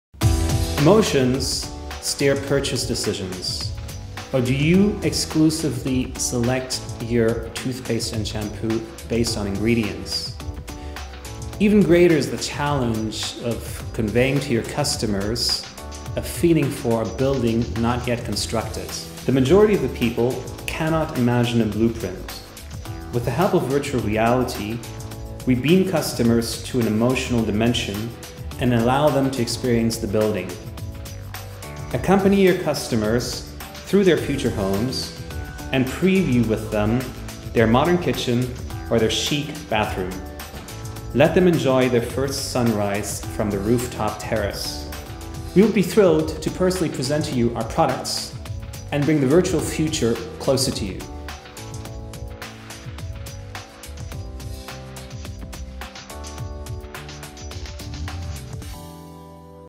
Deloitte Alumni - Martin Supancic y la realidad virtual
Martin Supancic y su startup VR- Immersive están aplicando la tecnología de realidad virtual inmersiva al mundo inmobiliario. El resultado es sorprendente.